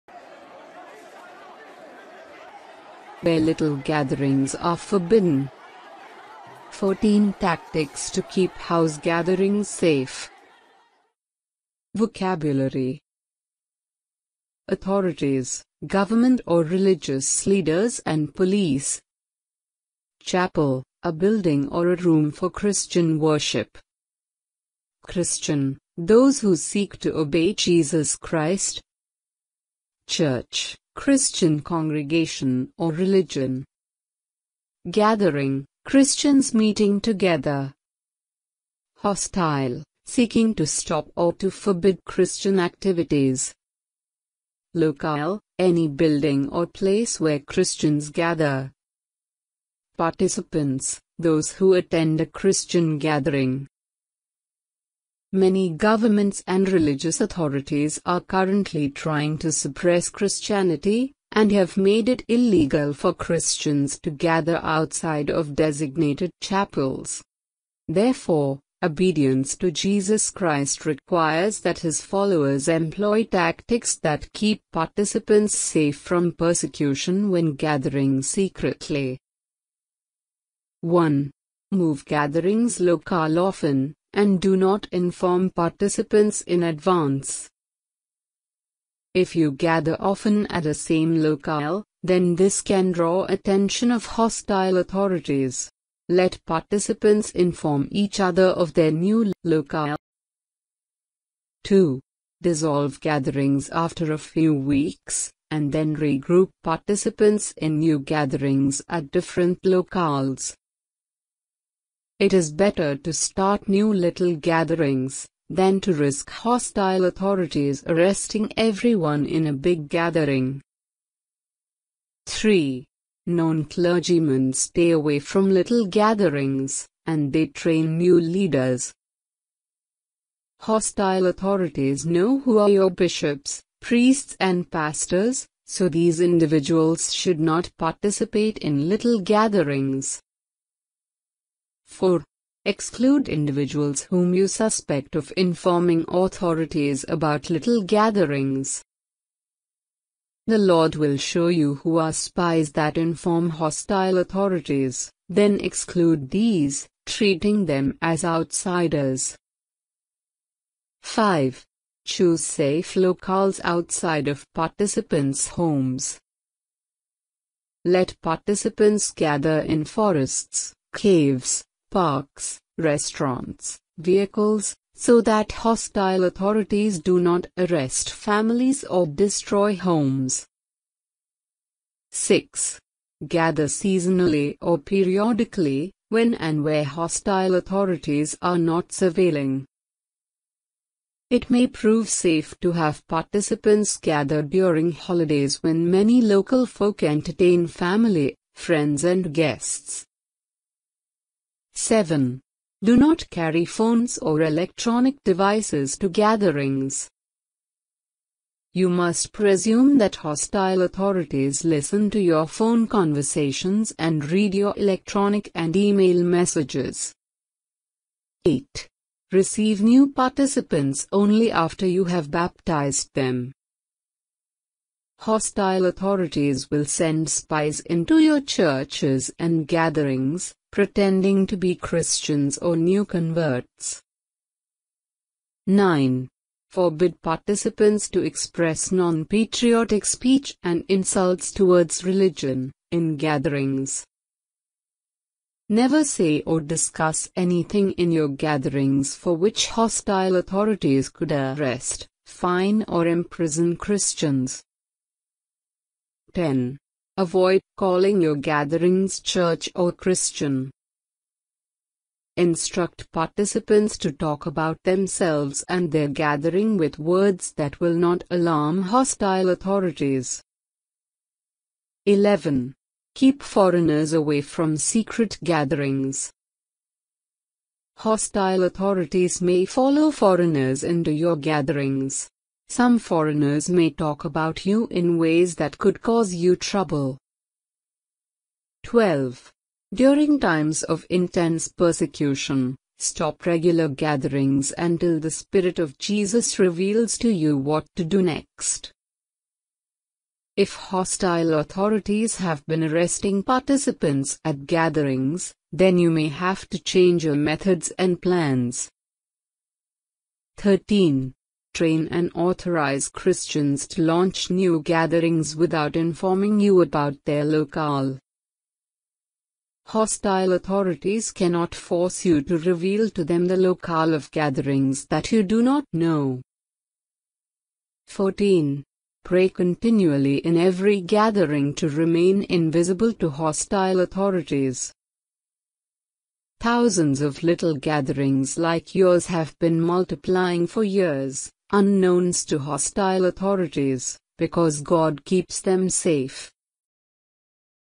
Wherever Little Gatherings Are Forbidden
14 tactics to keep house gatherings safe
paul-timothy.net December 2019

Vocabulary

Authorities : Government or religious leaders and police
Chapel : a building or a room for Christian worship
Christian : those who seek to obey Jesus Christ
Church : Christian congregation or religion
Gathering : Christians meeting together
Hostile : seeking to stop or to forbid Christian activities
Locale : any building or place where Christians gather
Participants : those who attend a Christian gathering

Many governments and religious authorities are currently trying to suppress Christianity, and have made it illegal for Christians to gather outside of designated chapels.

Therefore, obedience to Jesus Christ requires that his followers employ tactics that keep participants safe from persecution when gathering secretly.

1. Move gatherings’ locale often, and do not inform participants in advance.

If you gather often at a same locale, then this can draw attention of hostile authorities. Let participants inform each other of their new local.

2. Dissolve gatherings after a few weeks, and then regroup participants in new gatherings at different locales.

It is better to start new little gatherings, than to risk hostile authorities arresting everyone in a big gathering.

3. Known clergymen stay away from little gatherings, and they train new leaders.

Hostile authorities know who are your bishops, priests and pastors, so these individuals should not participate in little gatherings.

4. Exclude individuals whom you suspect of informing authorities about little gatherings.

The Lord will show you who are spies that inform hostile authorities, then exclude these, treating them as outsiders.

5. Choose safe locales outside of participants’ homes.

Let participants gather in forests, caves, parks, restaurants, vehicles, so that hostile authorities do not arrest families or destroy homes.

6. Gather seasonally or periodically, when and where hostile authorities are not surveilling.

It may prove safe to have participants gather during holidays when many local folk entertain family, friends and guests.

7. Do not carry phones or electronic devices to gatherings.

You must presume that hostile authorities listen to your phone conversations and read your electronic and e-mail messages.

8. Receive new participants only after you have baptised them.

Hostile authorities will send spies into your churches and gatherings, pretending to be Christians or new converts.

9. Forbid participants to express non-patriotic speech and insults towards religion, in gatherings.

Never say or discuss anything in your gatherings for which hostile authorities could arrest, fine or imprison Christians.

10. Avoid calling your gatherings “church” or “Christian.”

Instruct participants to talk about themselves and their gathering with words that will not alarm hostile authorities.

11. Keep foreigners away from secret gatherings.

Hostile authorities may follow foreigners into your gatherings. Some foreigners may talk about you in ways that could cause you trouble.

12. During times of intense persecution, stop regular gatherings until the Spirit of Jesus reveals to you what to do next.

If hostile authorities have been arresting participants at gatherings, then you may have to change your methods and plans.

13. Train and authorize Christians to launch new gatherings without informing you about their locale.

Hostile authorities cannot force you to reveal to them the locale of gatherings that you do not know.

14. Pray continually in every gathering to remain invisible to hostile authorities.

Thousands of little gatherings like yours have been multiplying for years, unknownst to hostile authorities, because God keeps them safe.